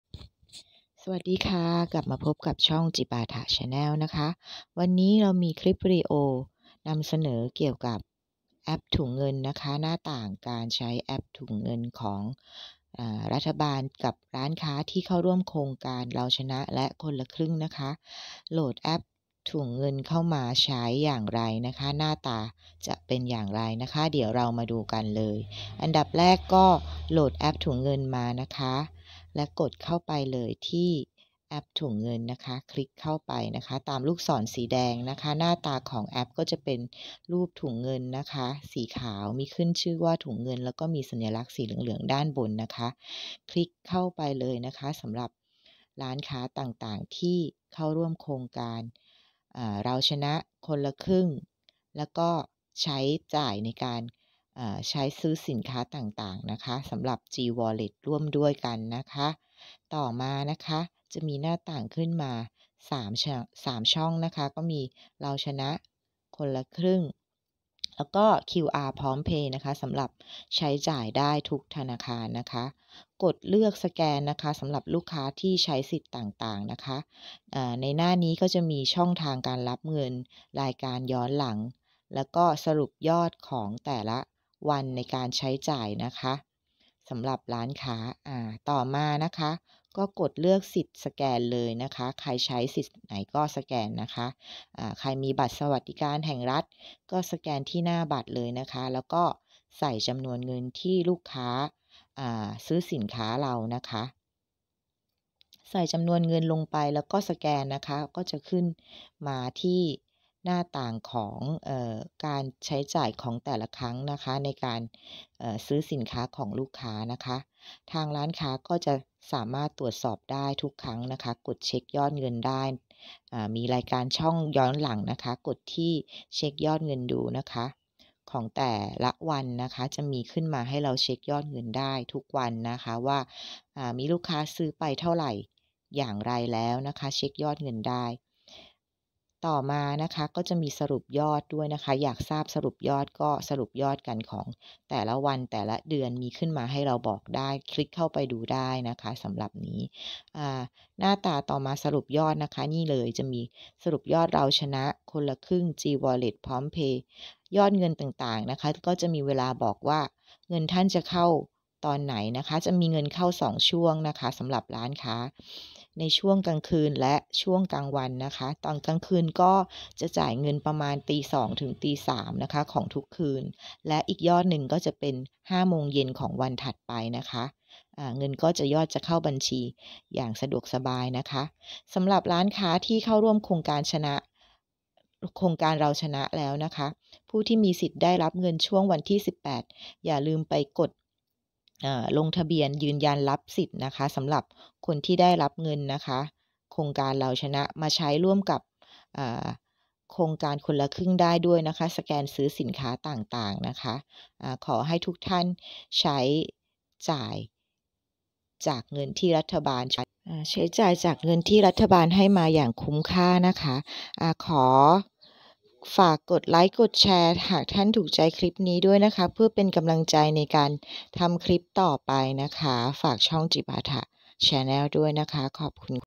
EP05.ถุงเงิน วิธีใช้แอปถุงเงินสำหรับร้านค้าโครงการรัฐบาล หน้าต่างการใช้งาน
EP5.ถุงเงิน หน้าตาแอปเข้าสแกนการชำระเงิน คนละครึ่ง เราชนะ และ gวอลเลทกระเป๋าเงิน